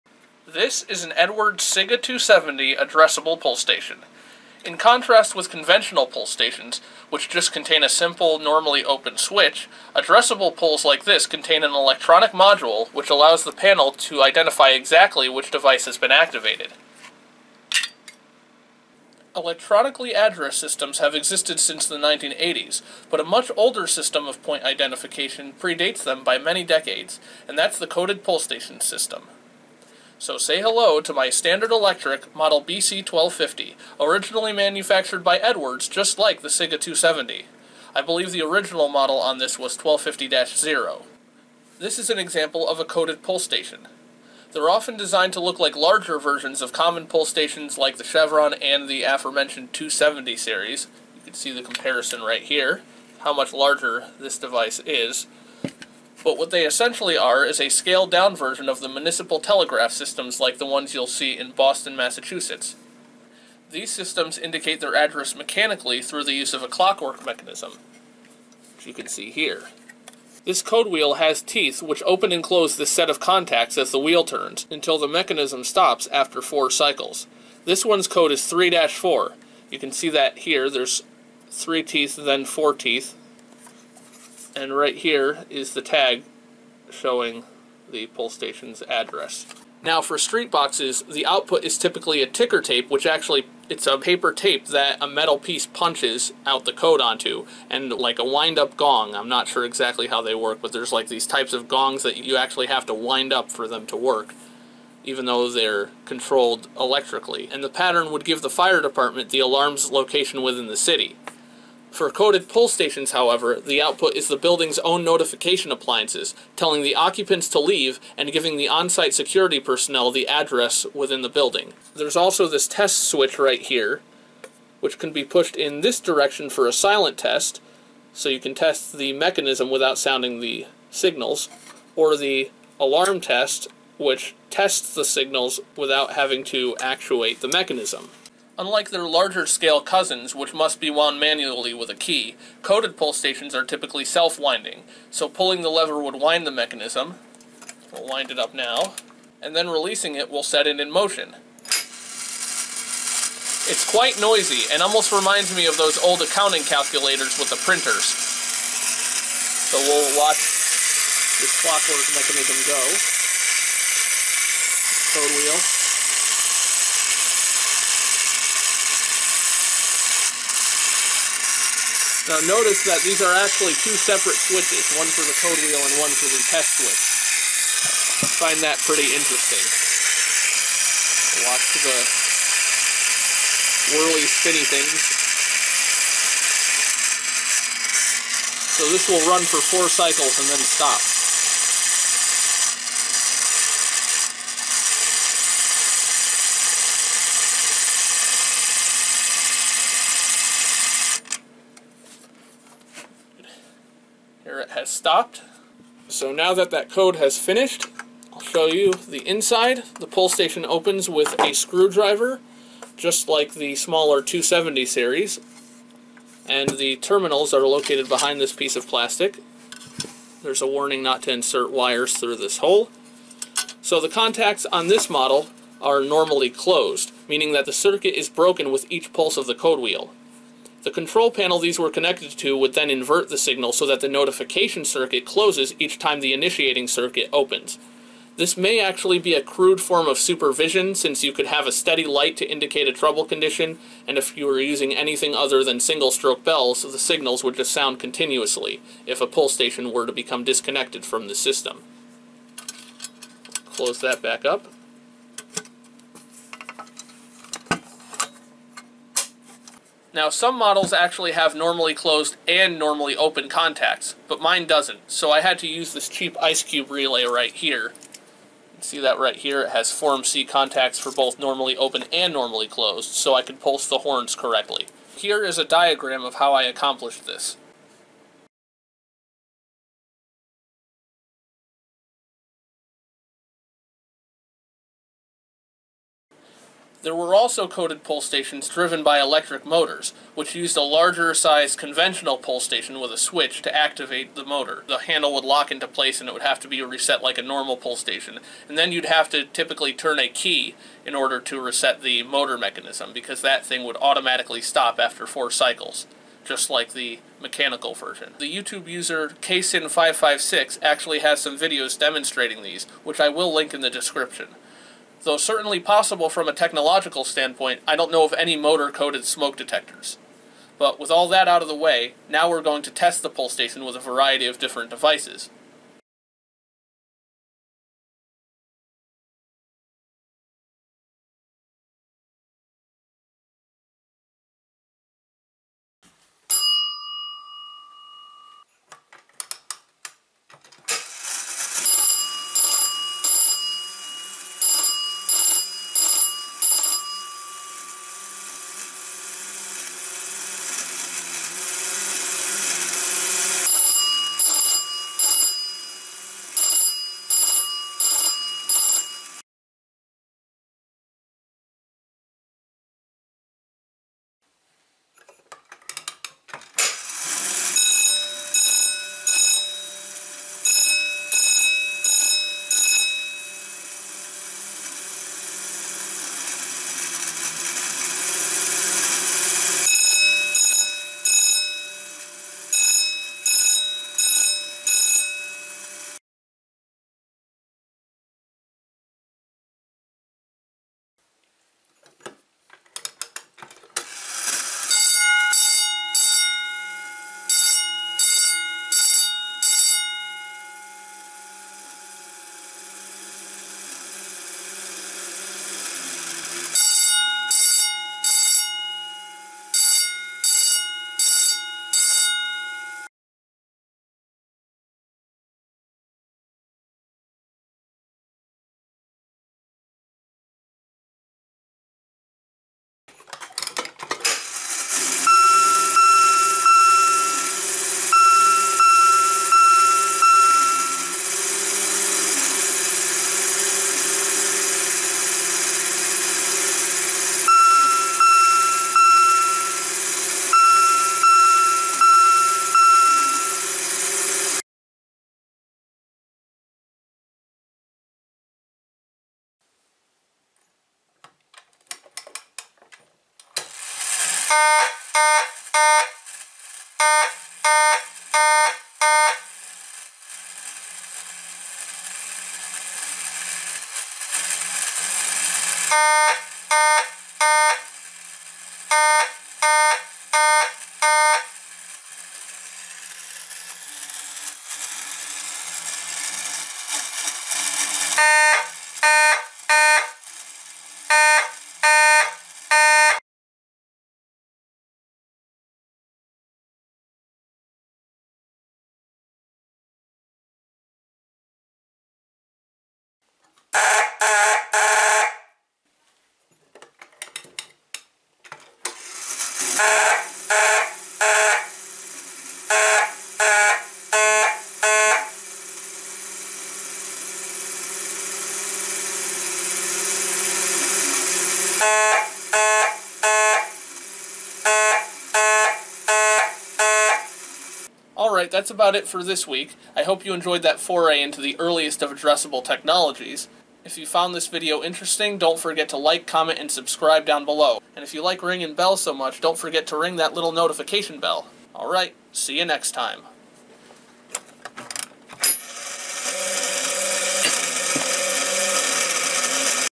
Standard Electric (Edwards) Coded Pull Station - Overview & Demo - Retro Fire Alarm Technology
In this video, I'll be showing you my vintage Standard Electric model BC-1250 coded pull station. This unit was originally manufactured sometime in the 1960's by Edwards. I'll be explaining how this device works, as well as demonstrating its use with a variety of bells, chimes, and mechanical horns.

Coded pull stations used a clockwork mechanism to pulse an audible code over the notification appliances, allowing building personnel to easily locate which station had been pulled. This long predates modern addressable systems, and is essentially a scaled-down version of the street box systems used in cities like Boston, Massachusetts.

Links:
- Simplex 4274-2 Coded Pull Station
- Honeywell W854 "Decimal Code Transmitter"

Timestamps:
0:00 - Intro
1:29 - Coded Pulls vs. Street Boxes
2:02 - Test Switch & Pulling the Lever
3:28 - Wiring, NO vs. NC contacts
4:46 - Motor-Coded Pull Stations
5:26 - Testing - 4" Bell
6:00 - Testing - 6" Bell
6:28 - Testing - 10" Bell
6:54 - Testing - Wheelock Chime
7:26 - Testing - Simplex 2903 + 9806
8:04 - Testing - Newer Simplex A/V
8:38 - Conclusion